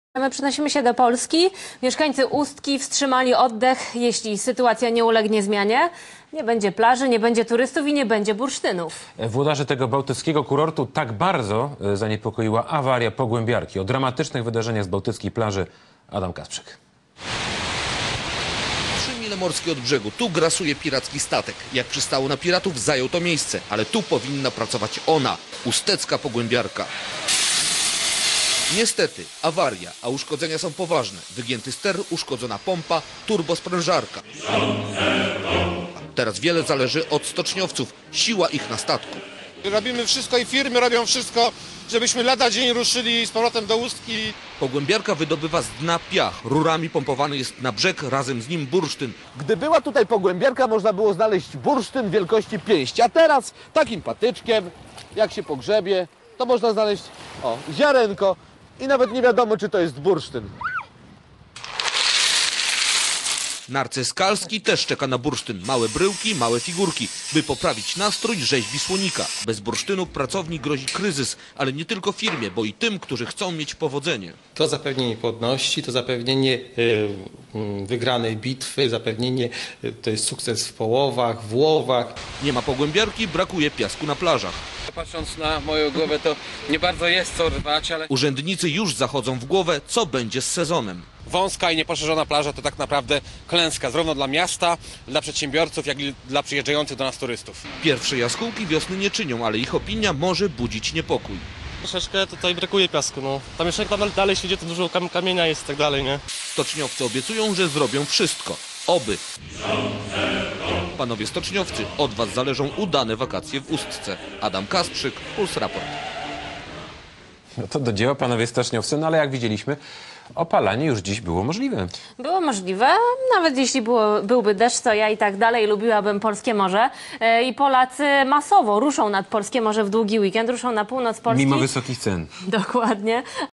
refulacja plaża w Ustce
O Ustce, plaży, bursztynie i Jacku który nie ma czego już rwać ;-)